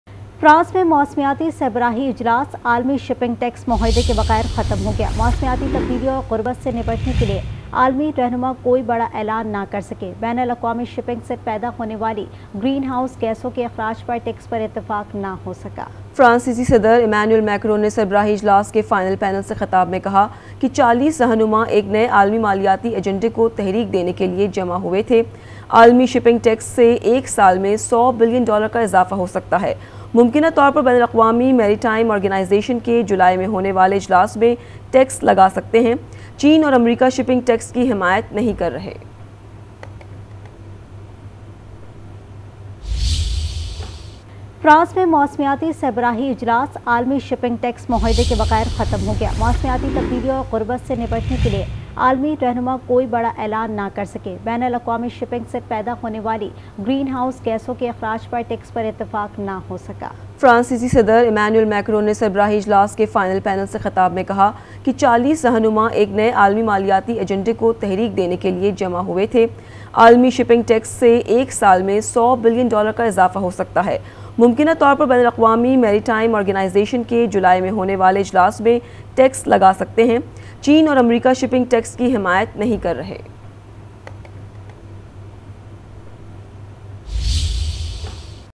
A climate summit in France ended without a global shipping tax agreement - Aaj News
A climate summit in France ended without a global shipping tax agreement - 24 June 2023 - Aaj News

Spot Light With Munizae Jahangir:
shorturl.at/gwFU6